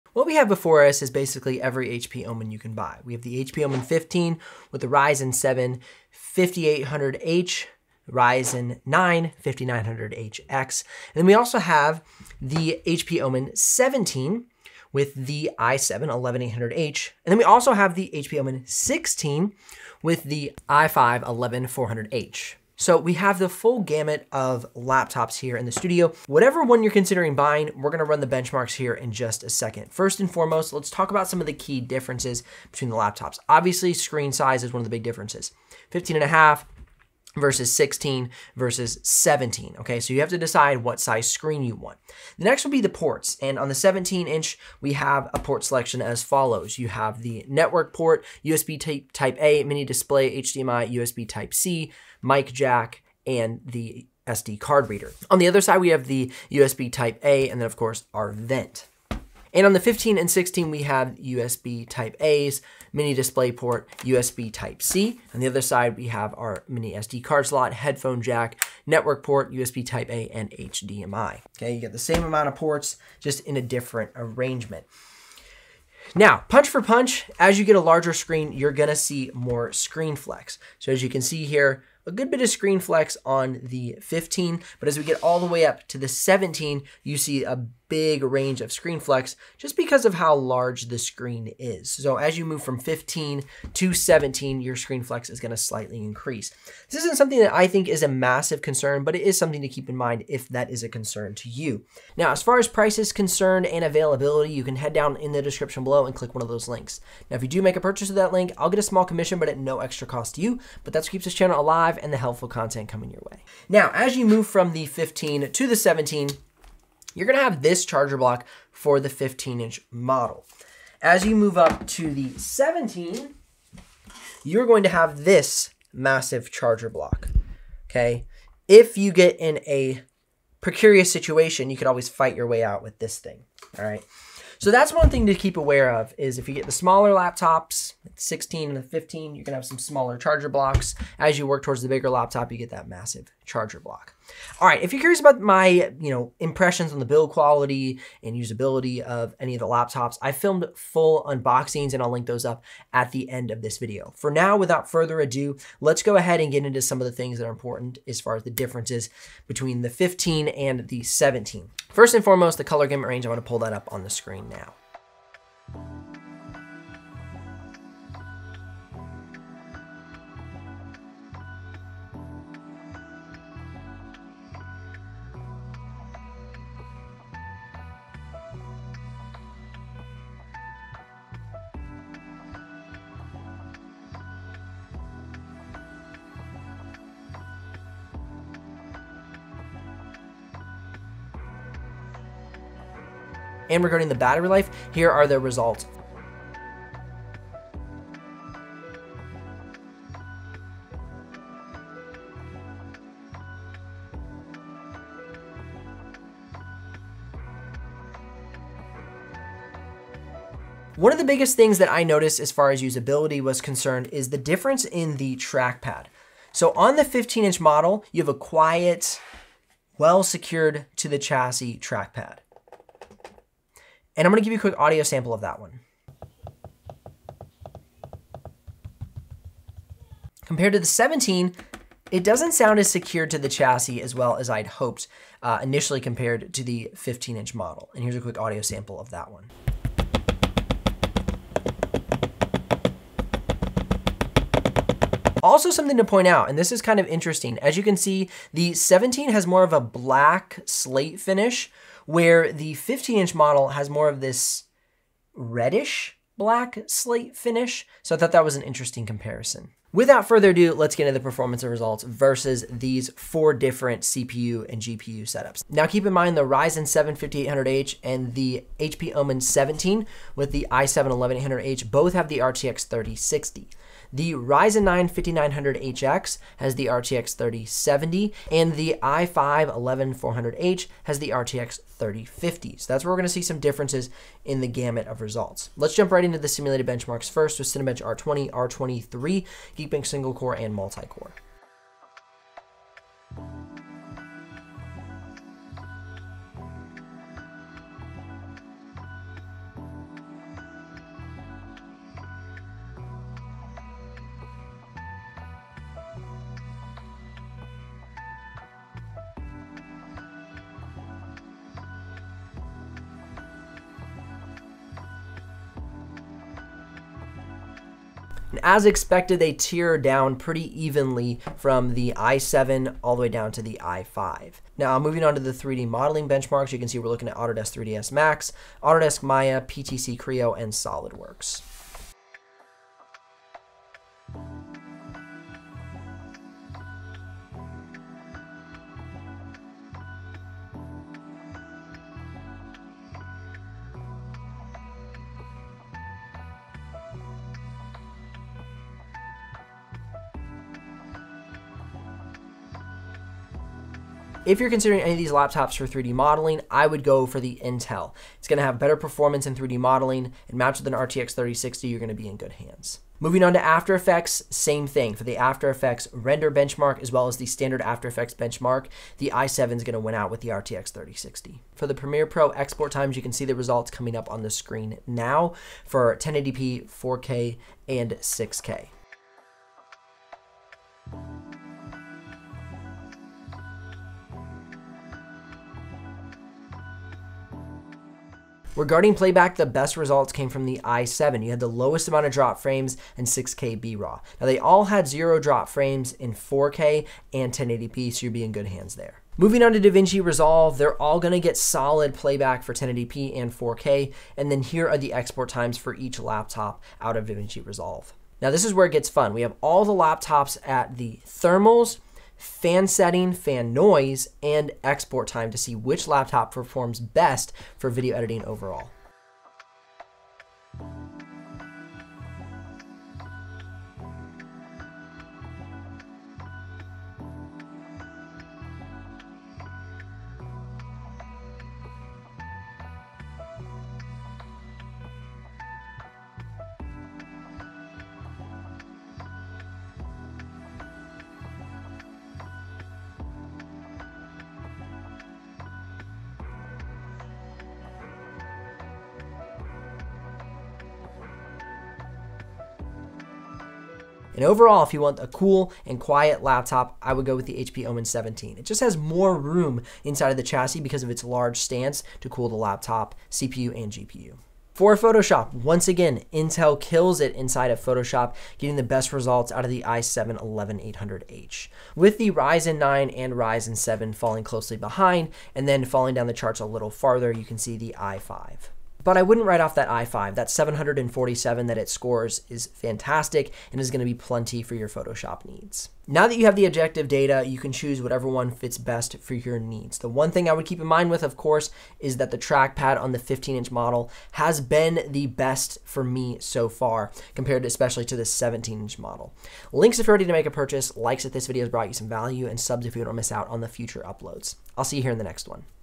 Which HP Omen Should You Buy // HP Omen Buyers Guide
In this video, we are going to look at the most common HP Omen laptops you can buy in order to help you decide which one is right for you.

💻 HP Omen 15 **Ryzen 7 5800H** - Commission Earned )
💻 HP Omen 15 **Ryzen 9 5900HX** - Commission Earned )

💻 HP Omen 16 **Ryzen** - Commission Earned )
💻 HP Omen 16 **Intel** - Commission Earned )

💻 HP Omen 17 ** Intel** - Commission Earned )

If you're looking for the right HP Omen for your needs you found the right video. In this video, we are going to look at each HP Omen to see how it handles 3D Modeling, After Effects, Video Editing in Premiere Pro and DaVinci Resolve, Photoshop, Battery Life and more.

Timestamps:
0:00 - HP Omen Buyers Guide
0:17 - Weight & Thickness
0:27 - What are the differences
0:43 - I/O Ports
1:17 - Screen Flex
1:42 - Pricing and Availability
1:53 - Charger Block Size
2:43 - Color Gamut Range & Color Accuracy
3:13 - Battery Life
3:31 - Trackpad
4:20 - Performance Benchmark Results
4:47 - Cinebench R20 & R23
5:09 - Geekbench Single & Multi-Core
5:26 - Autodesk 3DS Max & Autodesk Maya
5:46 - PTC Creo & Solidworks
6:03 - After Effects Benchmark
6:12 - Premiere Pro Export Times
6:29 - 4k Premiere Pro Playback Test
6:39 - DaVinci Resolve Export Times
4:48 - Thermal Results
7:44 - Photoshop Benchmark Results
8:06 - Which one should you buy?

💻 HP Omen 15 Spec Sheet for the model I reviewed:
CPU: AMD Ryzen 7 5800H (8 Cores 16 Threads)
GPU: NVIDIA RTX 3060 VRAM 6GB
RAM: 16GB @ 3200Mhz
Storage: 512GB

💻 HP Omen 15 comes with:
CPU: Ryzen 9 5900HX (8 Cores 16 Threads)
GPU: NVIDIA RTX 3070 8GB VRAM
RAM: 16GB @ 3200Mhz
Storage: 1TB

💻 HP Omen 16 i5-11400H Specs:
CPU: i5-11400H (6 Cores 12 Threads)
GPU: NVIDIA RTX 3050 4GB VRAM
RAM: 16GB @ 3200Mhz
Storage: 512GB
Weight: 5.44 Lbs

💻 HP Omen 17 Specs:
CPU: Intel Core i7-11800H (8 Cores 16 Threads)
GPU: NVIDIA RTX 3060 6GB VRAM
RAM: 16GB @ 3200Mhz
Storage: 512GB